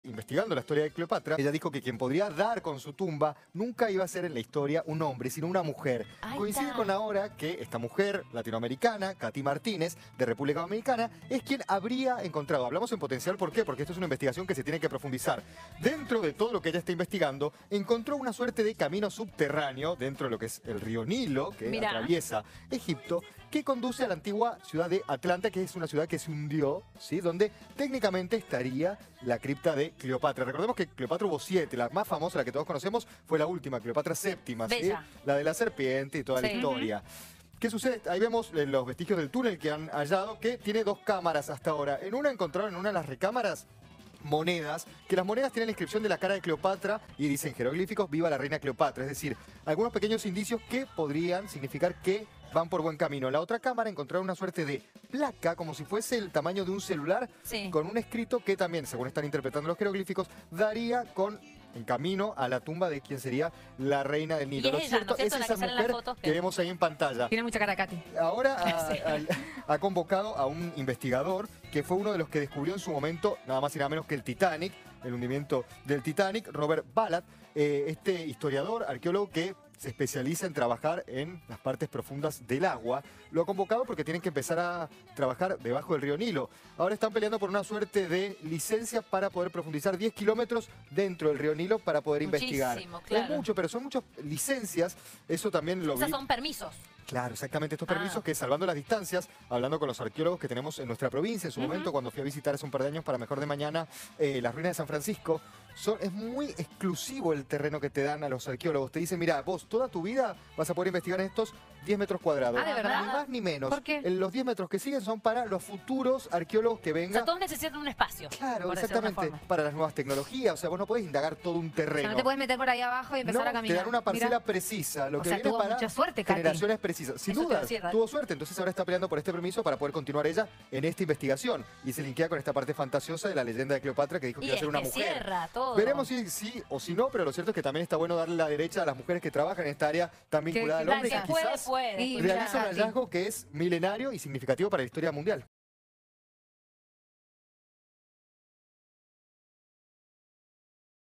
🤩 El hallazgo de la arqueóloga latinoamericana que lleva 20 años buscando la tumba de CLEOPATRA
A lo largo de décadas, varios equipos han intentado encontrarlo, desde el siglo XIX, lo que llevó a figuras importantes como Napoleón a sentirse desilusionadas.
Sin embargo, la determinación y el optimismo de la arqueóloga Kathleen Martínez, finalmente llevaron al hallazgo completo del templo.